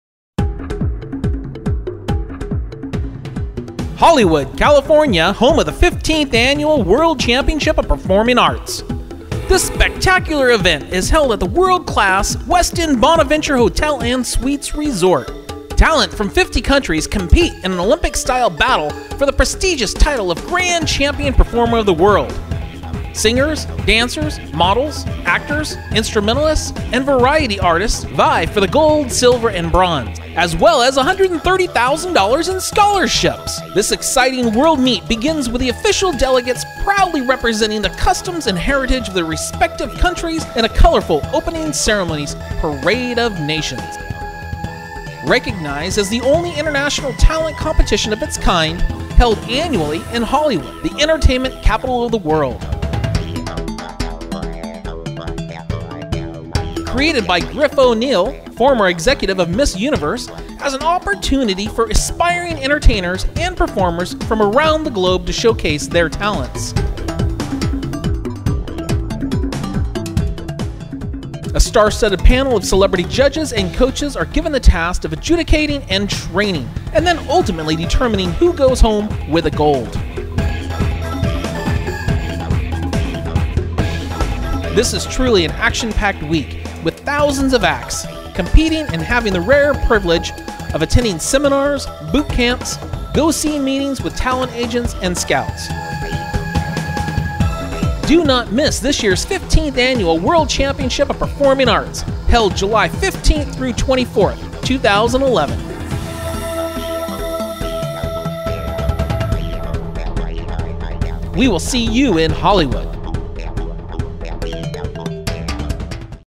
World Championships of Performing Arts (WCOPA) 2010 Promo (Featuring Gilbert Gonzalez Jr)
Gilbert Gonzalez Jr is featured here in the promo video for the Olympics of performing: The World Championships of Performing Arts 2010, where he was a 5X Silver Medalist and Semi-Finalist World Champion.